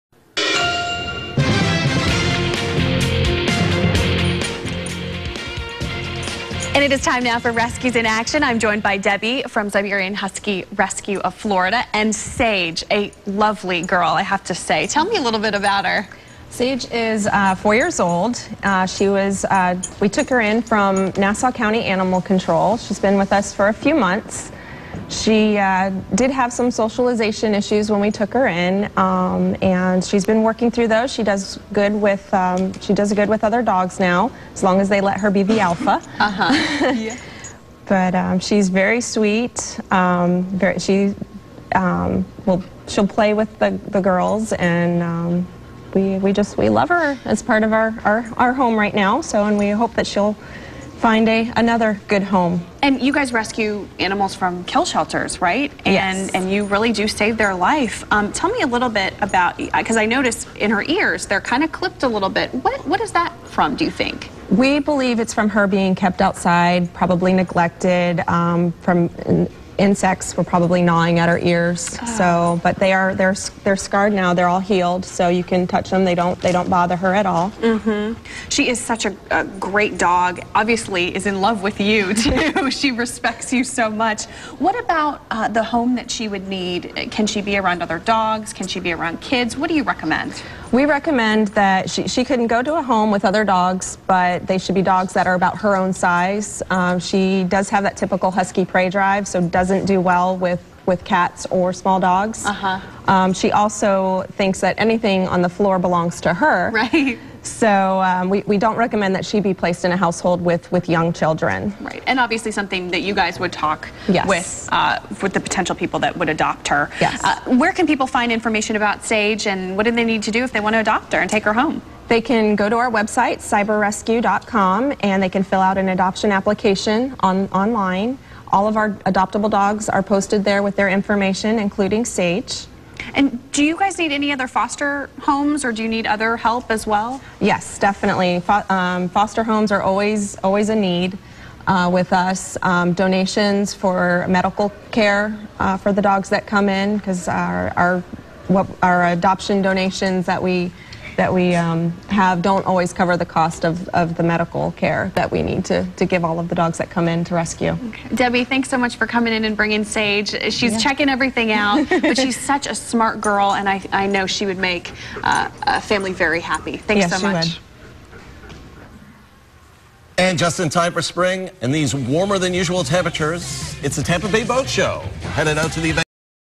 Rescues in Action - Sage
Meet Sage - a lovely Siberian Husky in need of an adoptive human family.